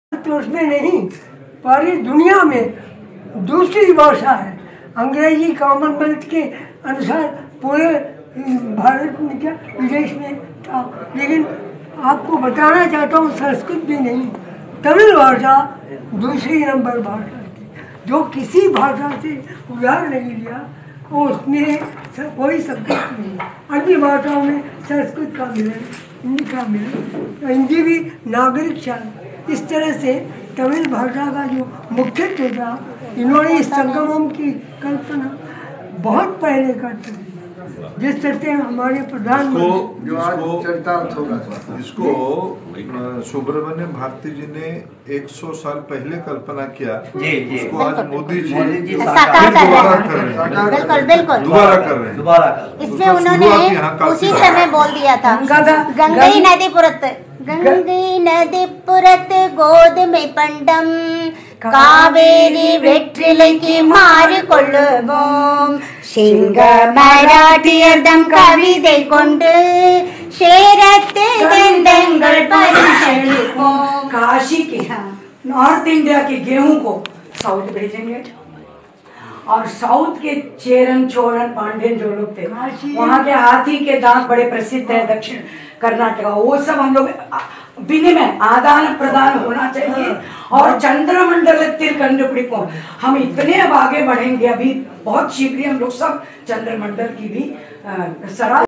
মহাকবি সুব্রহ্মণ্যম ভারতীর বাসস্থান শিক্ষার কেন্দ্র এবং পবিত্র একটি তীর্থভূমি
আজ কাশীতে মহাকবি সুব্রহ্মণ্যম ভারতীর ৯৬ বছর বয়সী ভাইপো শ্রী কে ভি কৃষ্ণন এবং তাঁর পরিবারের সাথে দেখা করার সৌভাগ্য হয়েছিল।

সর্বকালের সর্বশ্রেষ্ঠ তামিল সাহিত্যিকদের মধ্যে একজন, কাশীর হনুমান ঘাটে অবস্থিত মহাকবি ভারতীর বাসস্থান শিক্ষার কেন্দ্র এবং পবিত্র একটি তীর্থভূমি।